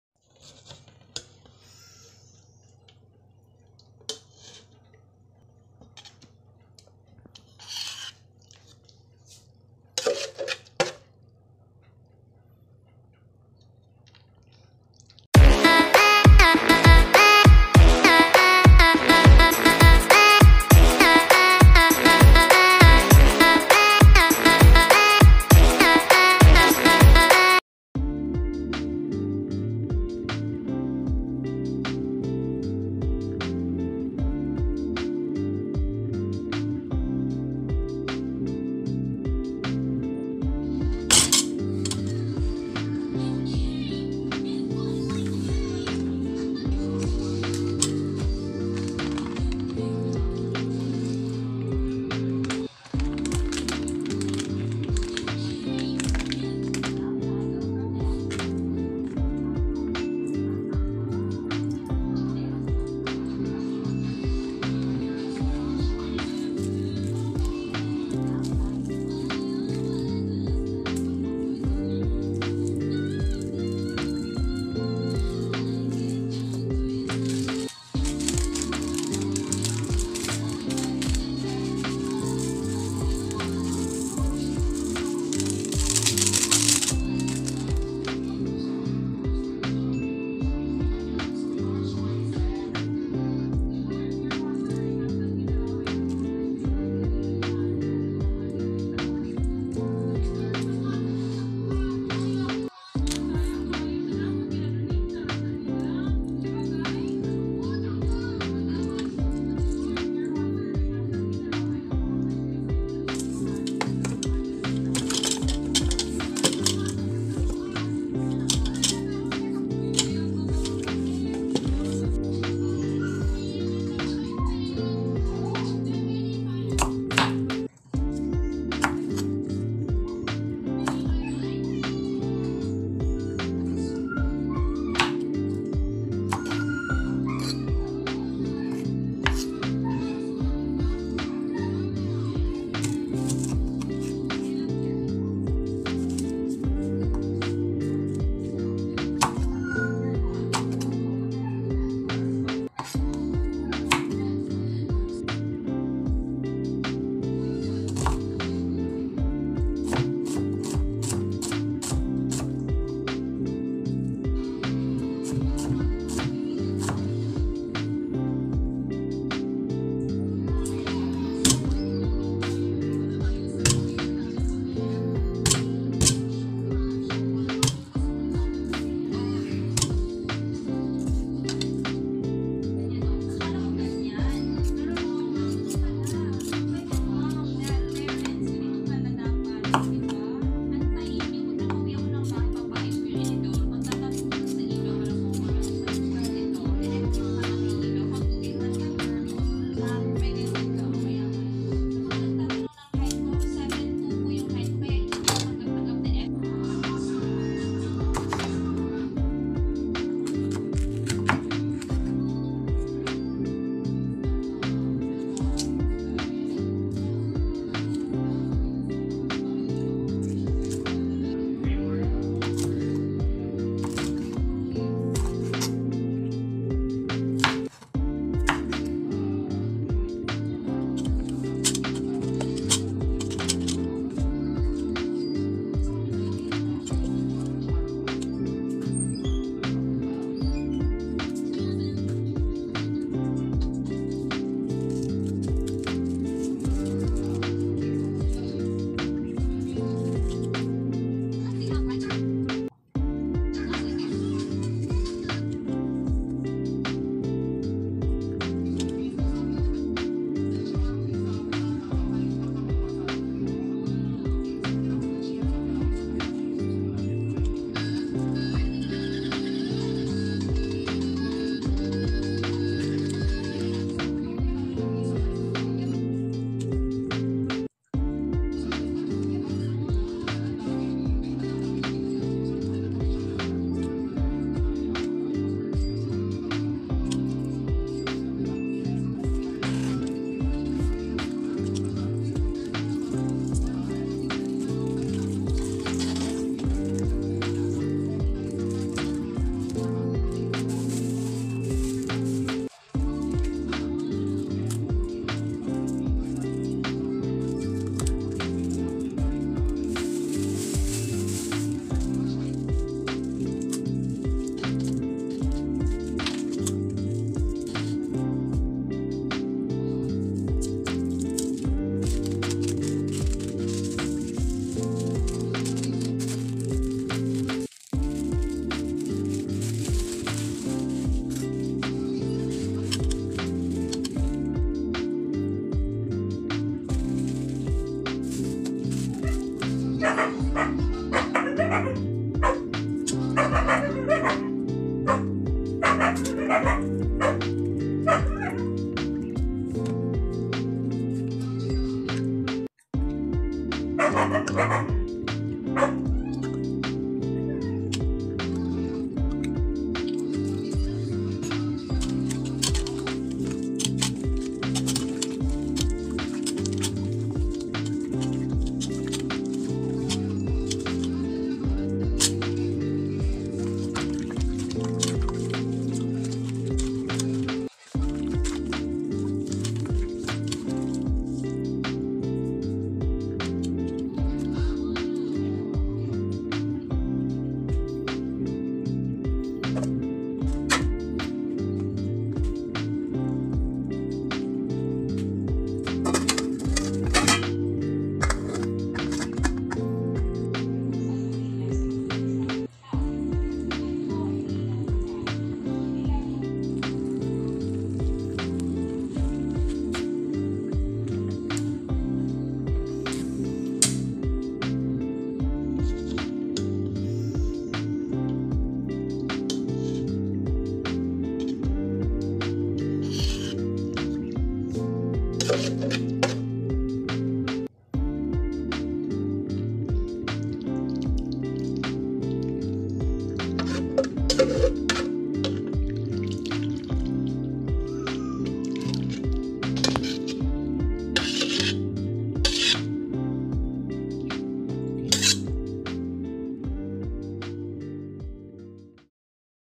Improvising my Mother's Recipe for Pinaupo na Manok
Wazzup guys. Matagal² din di naka post.
Here's my Own Recipe for pinaupo na manom. Mas masarap parin sa nanay ko. hahaha

Recipe:
Whole Chicken
Soy Sauce
Vinegar
Ketchup
Sprite or Coke
Salt
Sugar
Onion
Spring Onion
Garlic
Ginger
Ginger Grass
Bell Pepper
Pepper Powder
Calamansi